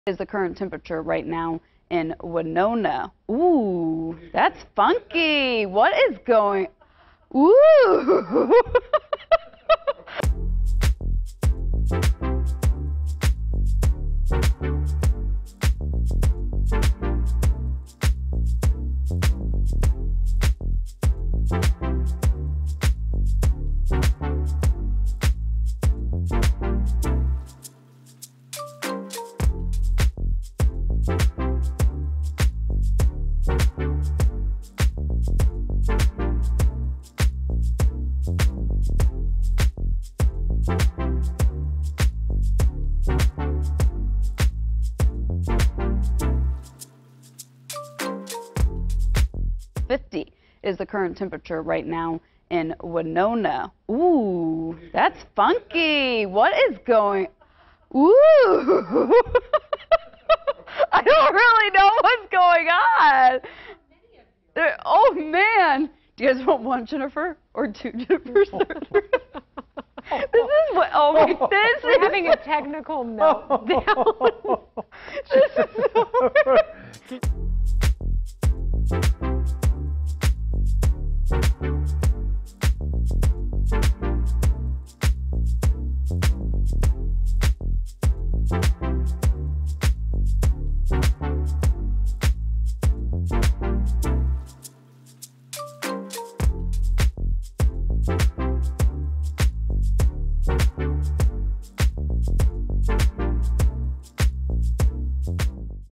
Surprise par un bug vidéo, cette présentatrice météo s'est bien amusée
Le bulletin météo du mardi 11 mai de la chaîne Fox 9 Minneapolis a été marqué d'un incident vidéo. Plutôt que de paniquer, la présentatrice a décidé de s'en amuser.